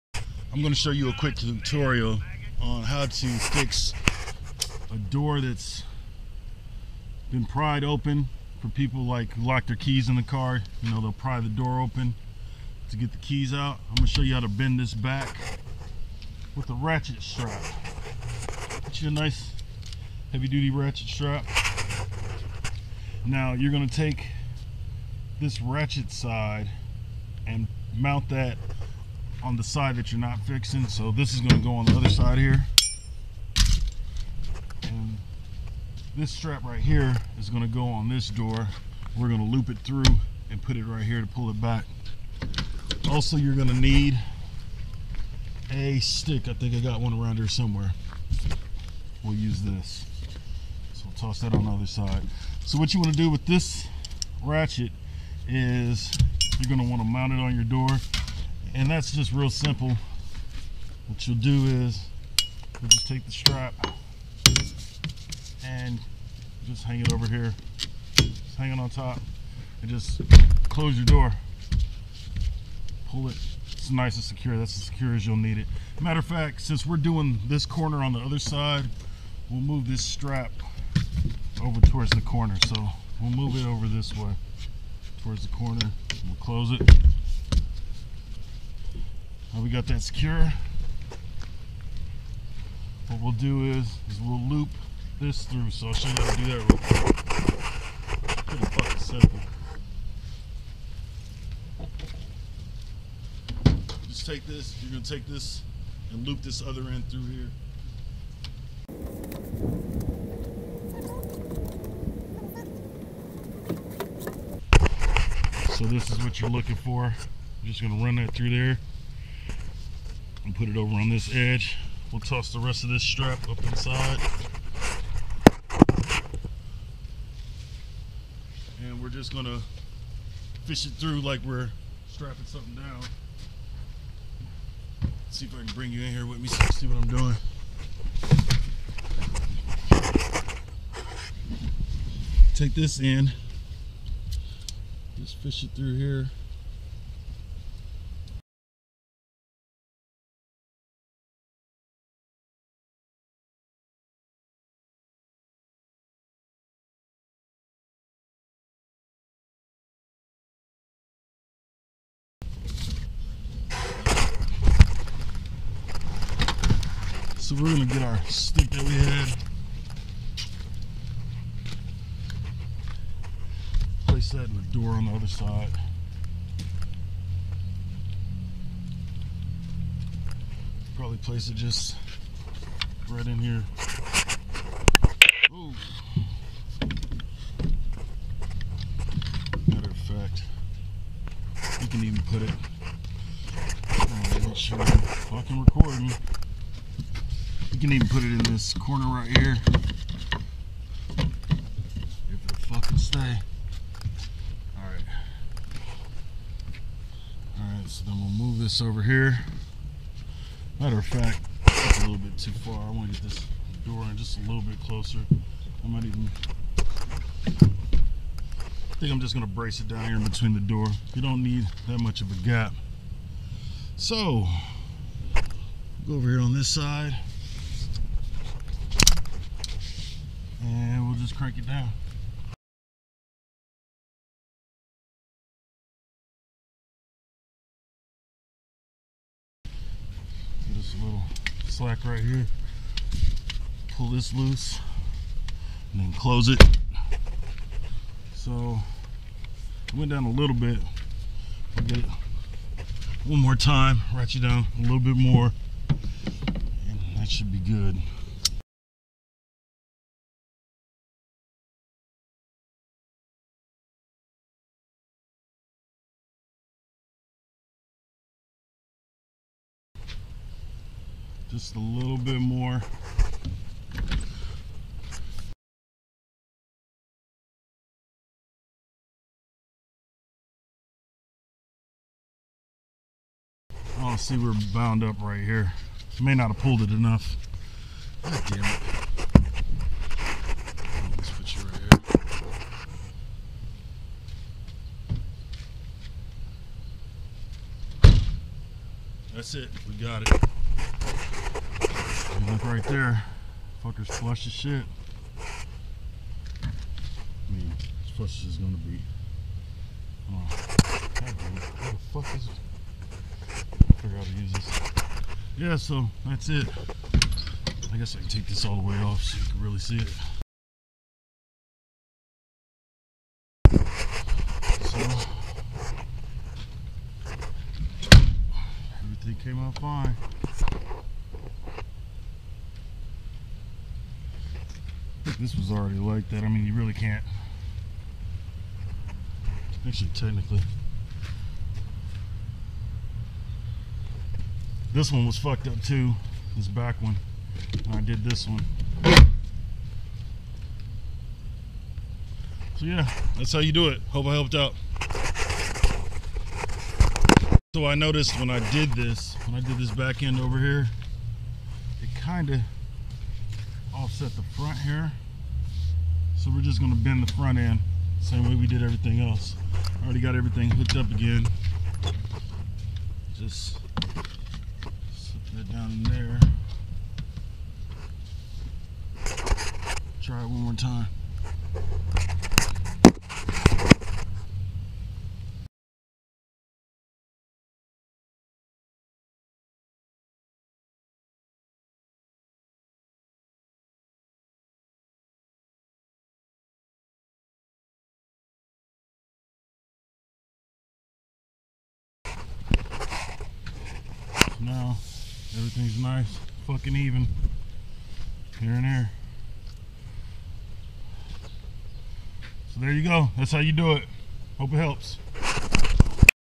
How to fix a car door that's been pried open.
I'm not a mechanic but this is how I fixed my Jeep door. I found that it's better to bend the door a little at a time to ensure that you don't over bend your door.